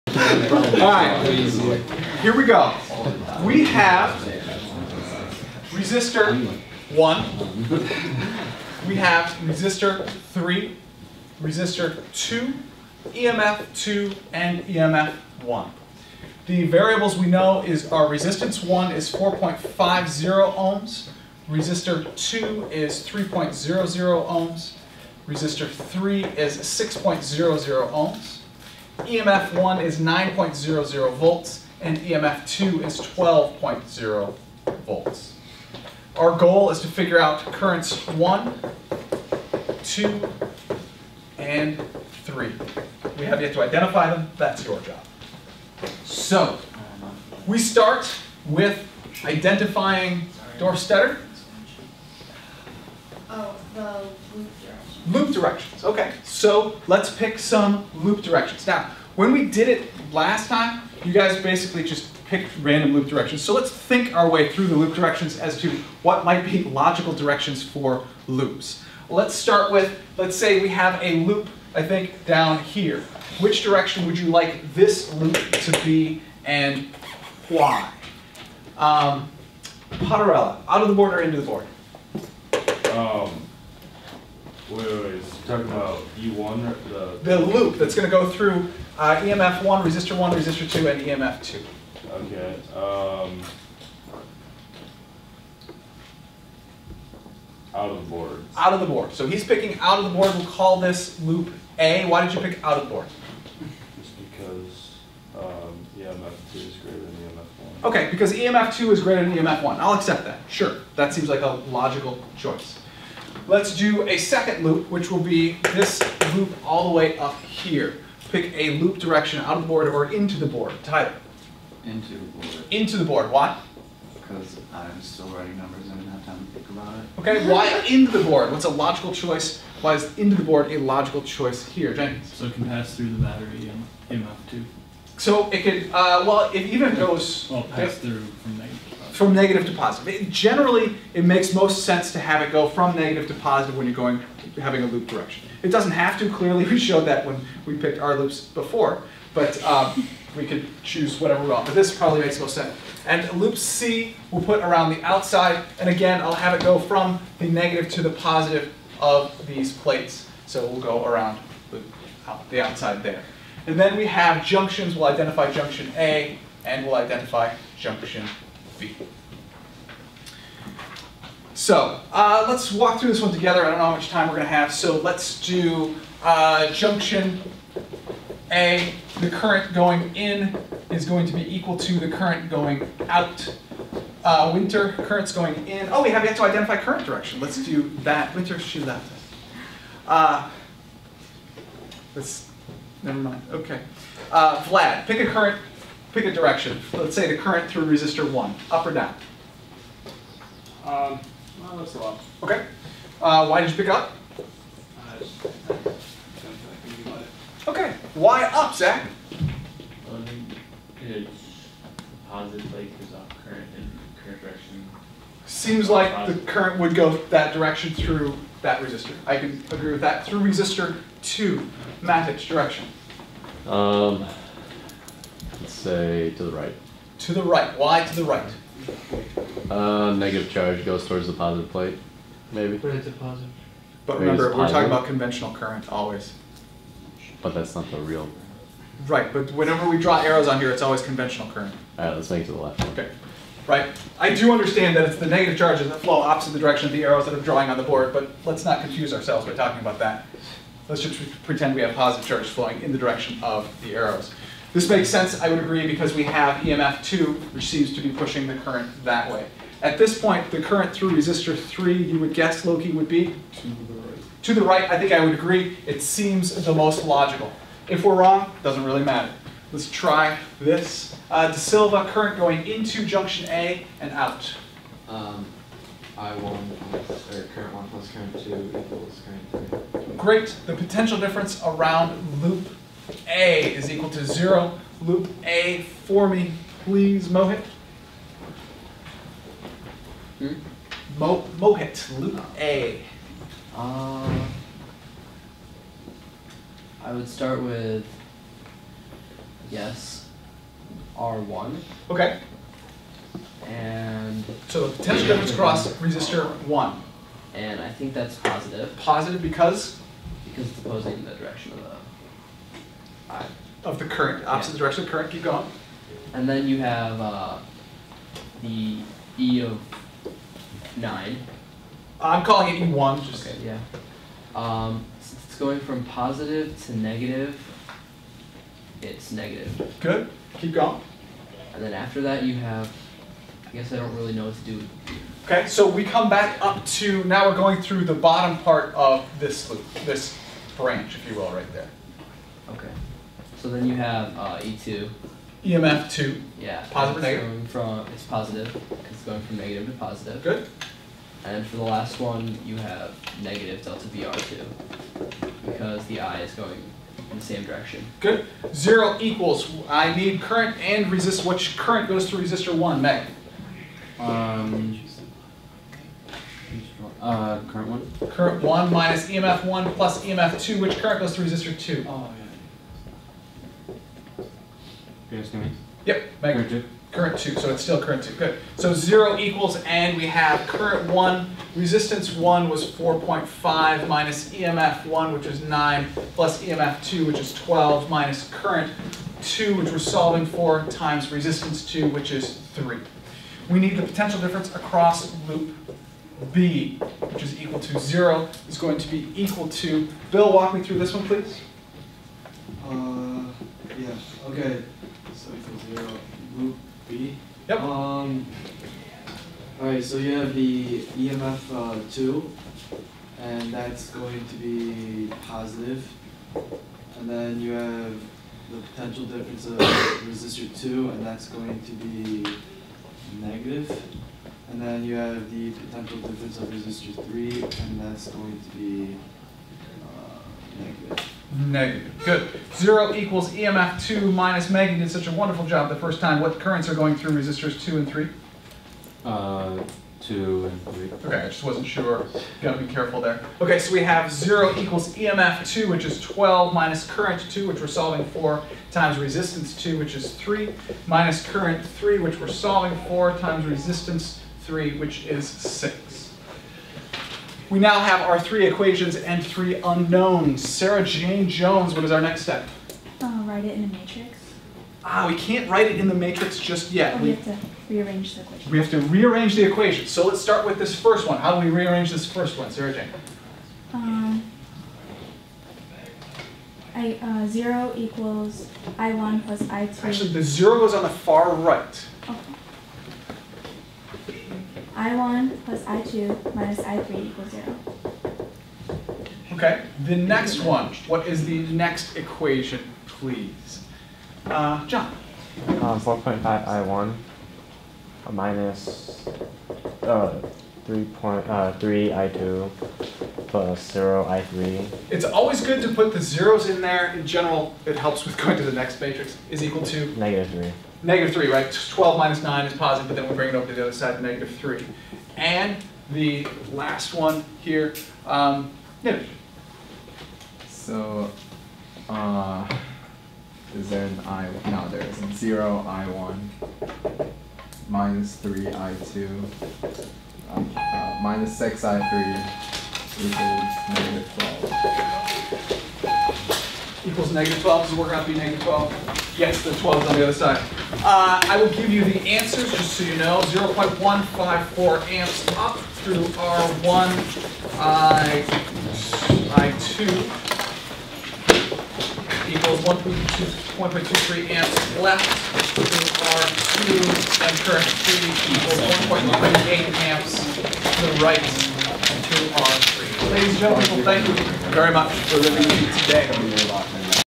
Another Kirchhoff's Rules Example Problem - AP Physics C
AP Physics C Another Kirchhoff's Rules Example Problem. has Lecture Notes, Groupings and Sequencing of my lecture videos.